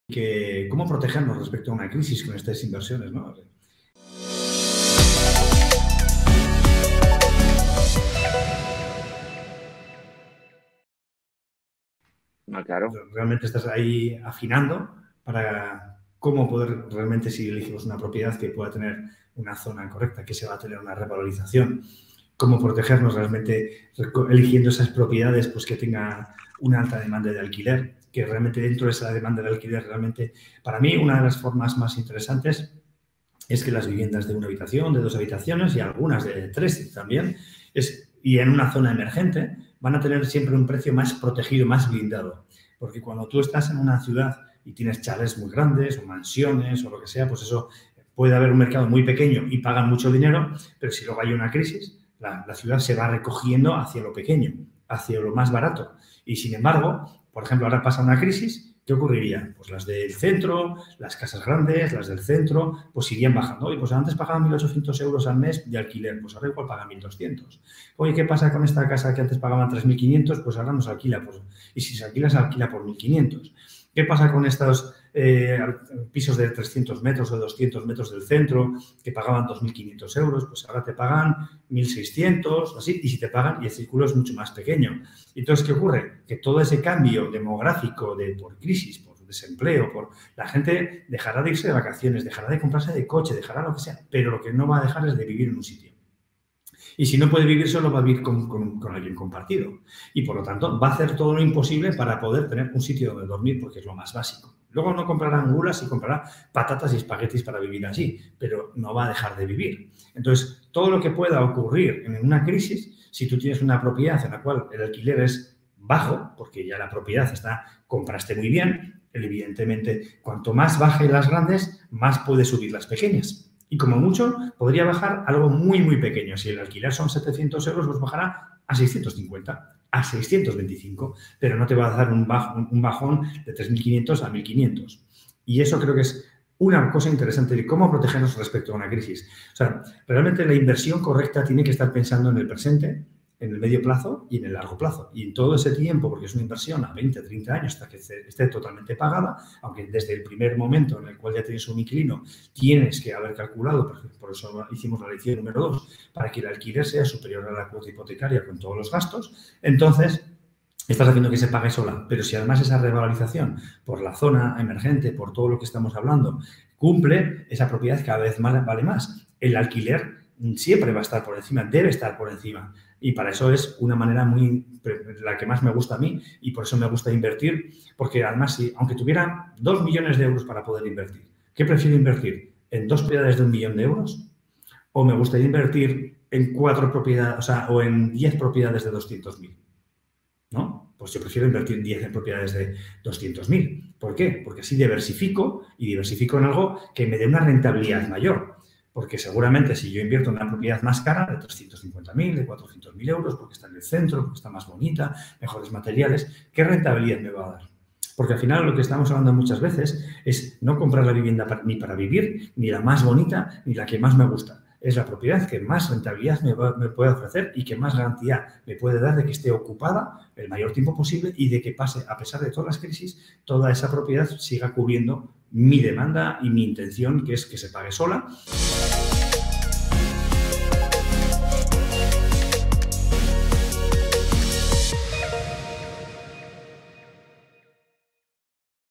¿CÓMO PROTEGEMOS UNA INVERSIÓN ANTE UNA CRISIS? | BrokersDigitalesIberia.com
En este video te contamos cuáles son los factores claves que debe tener tu inversión para evitar caer en una crisis.